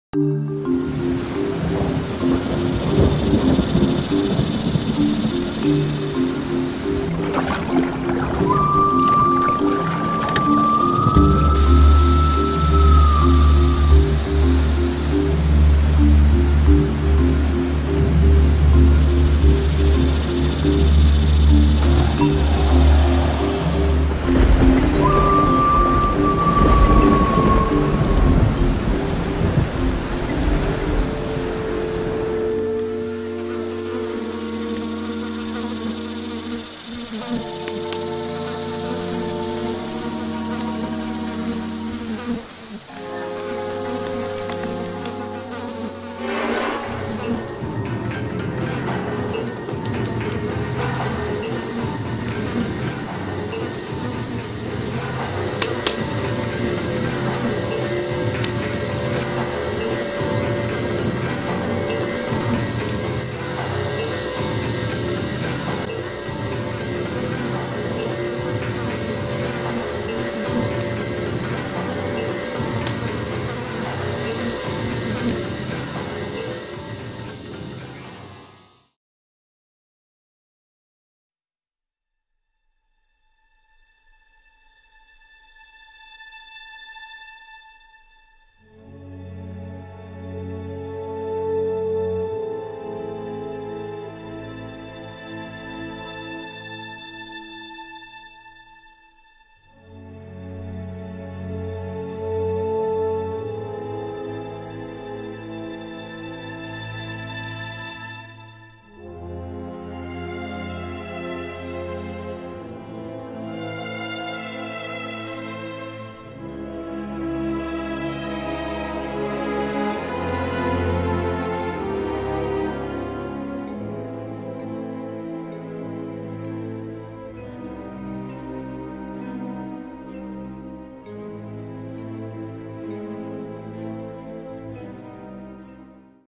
the end of the Carboniferous period (claymation)
tell me if there is one prehistoric animal that you want to see🦕.
what are your favourite dinosaurs or other animals of prehistory.